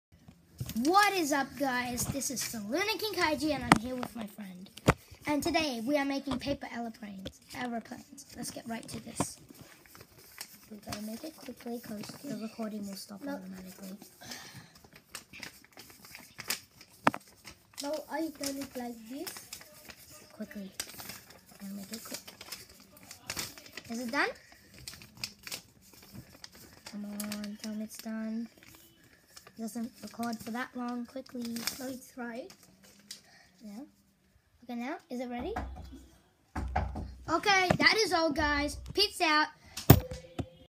Making paper aeroplanes with my friend!!!!
He is really talented to do it so perfectly.